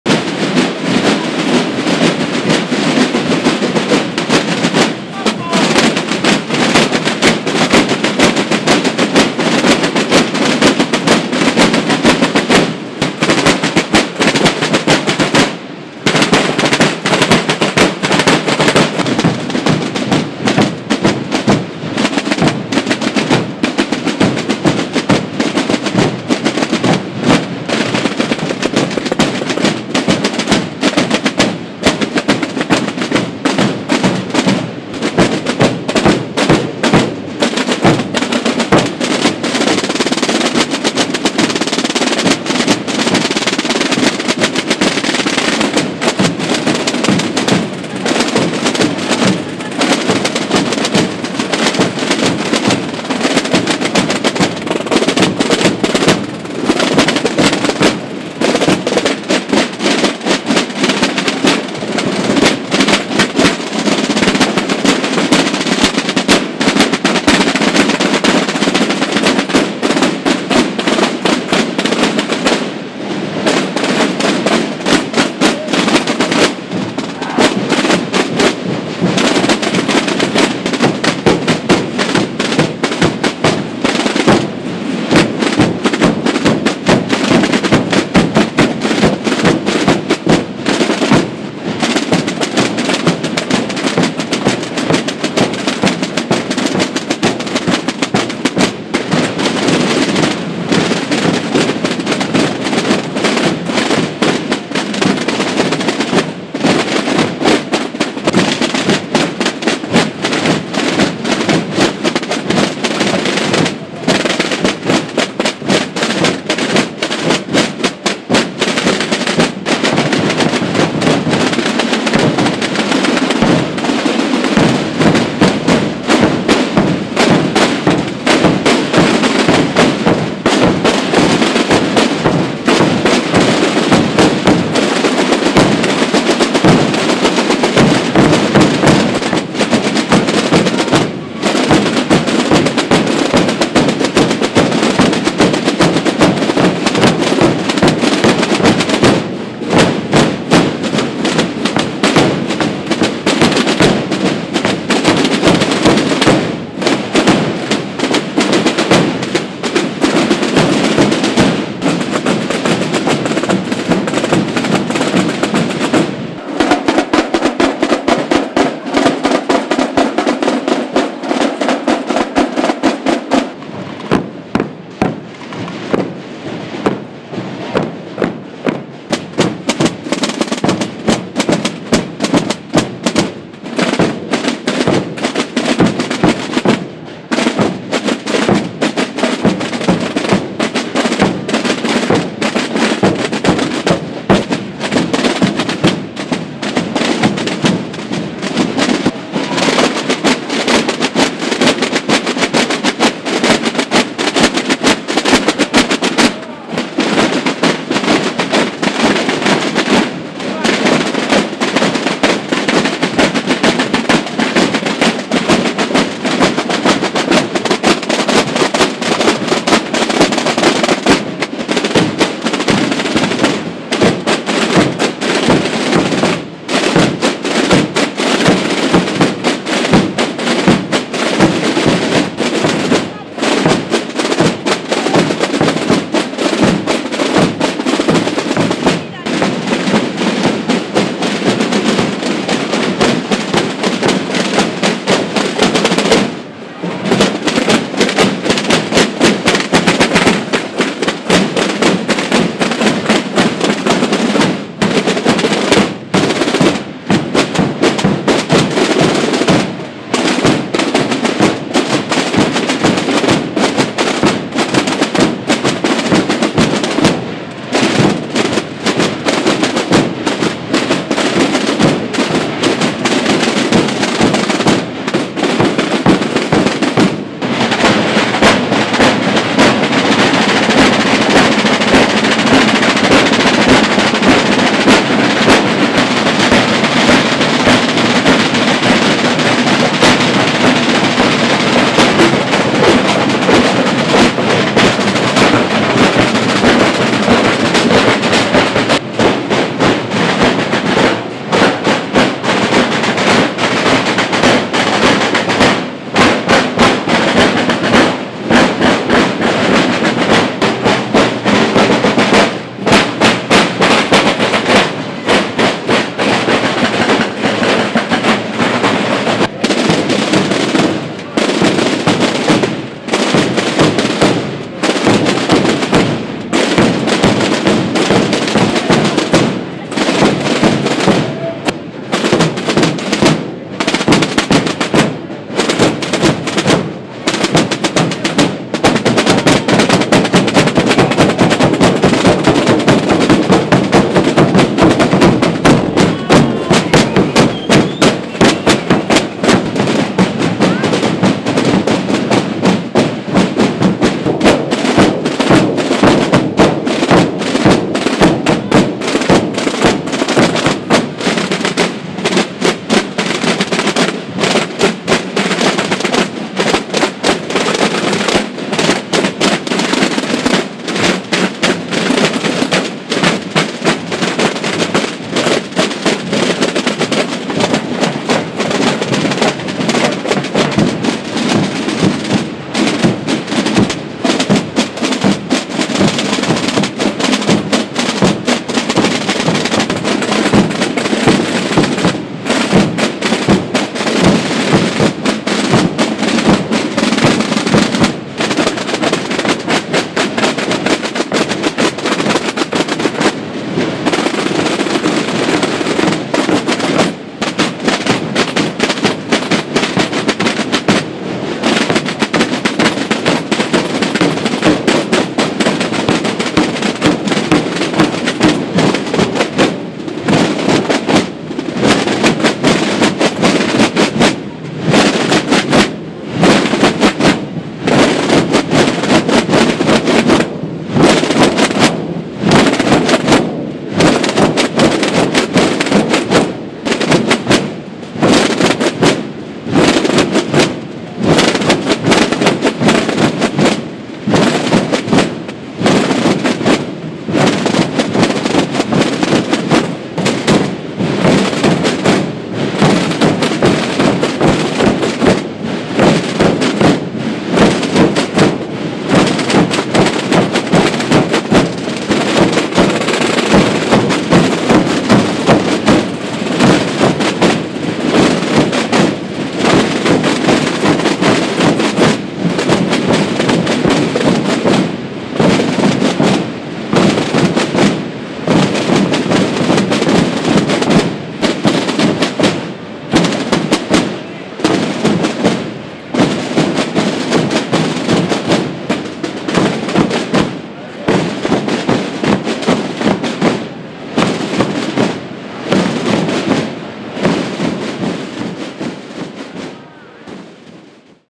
La Comarca.tv - Desfile Jornadas Nacionales Tambor Calanda
Desfile de todas las poblaciones participantes en las XXIX Jornadas Nacionales de Exaltación del Tambor y el Bombo celebradas en Calanda.